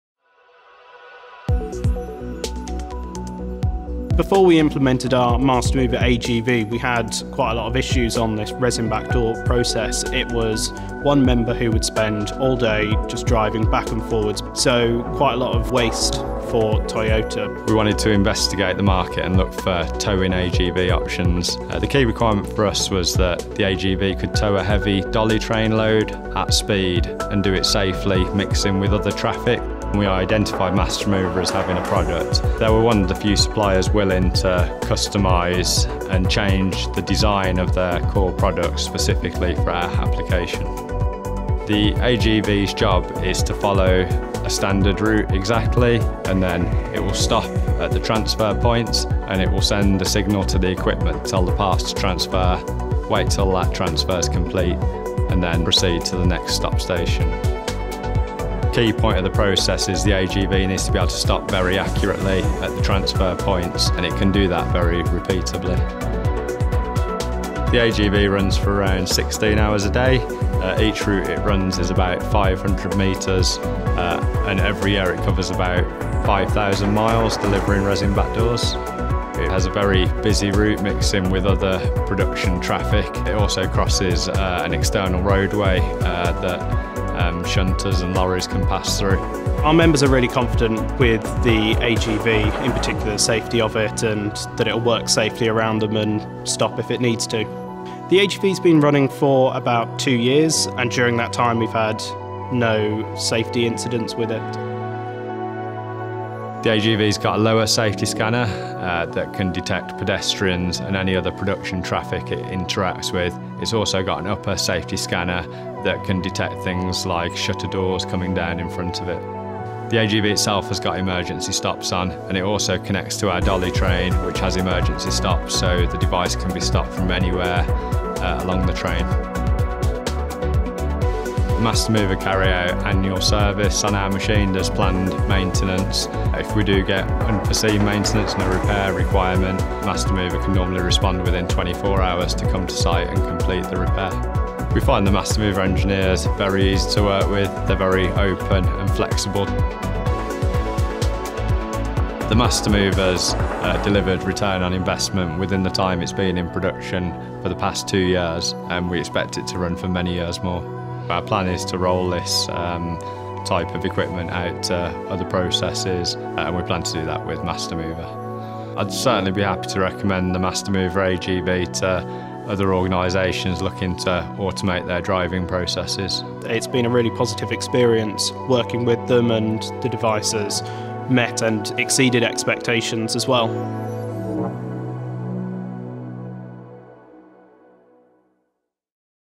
Automated Guided Vehicle ► DRIVES EFFICIENCY at Toyota Motor Manufacturing [AGV Systems]
Industrial automation and Automated Guided Vehicles greatly improves efficiencies at Toyota Motor Manufacturing in the UK.

Automated Guided Vehicles (AGVs) have the ability to be transformational when it comes to driving operational efficiency within industrial manufacturing. Featuring best-in-class navigation and safety technologies, AGVs remove the need for human operation, carrying out movement of products, equipment and materials on time, every time.

This not only makes the operation more efficient but protects your people from the stresses and injuries associated with manual handling, or higher risk load moving solutions. Furthermore, it also means your people can focus on tasks of greater value, driving additional ROI and efficiency.

BLOGS:

► What are Automated Guided Vehicles?

► How to: Buying an AGV, the key steps you need to know